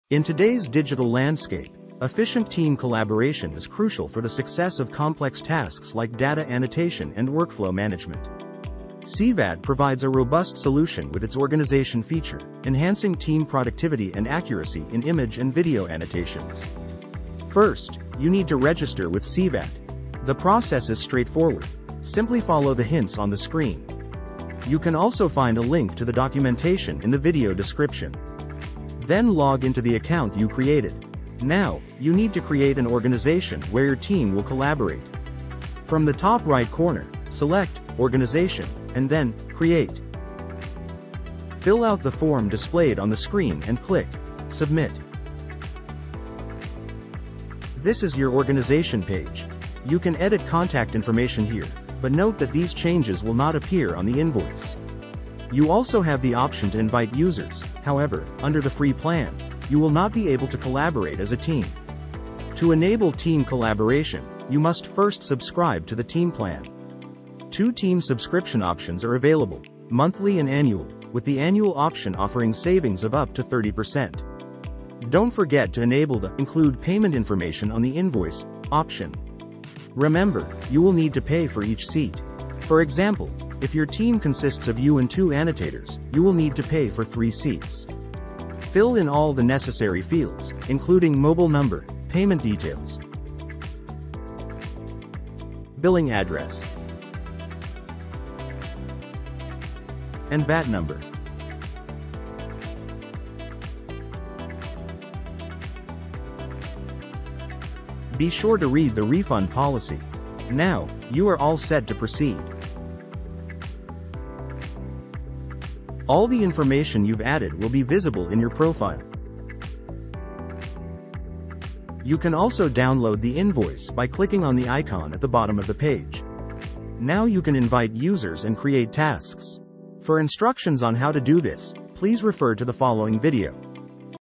CVAT for Organizations and Teams: Create Organization and Subscribe to Team Plan
0:00   Introduction
0:17 - Registering with CVAT
0:31 - Creating an organization
0:47 - Organization page overview
1:07 - Subscription options (monthly and annual)
1:14 - Including payment information on invoices
1:19 - Payment details and seat costs
1:55 - Viewing Organization information
2:01 - Downloading invoices

Not a CVAT.ai user?

Happy annotating!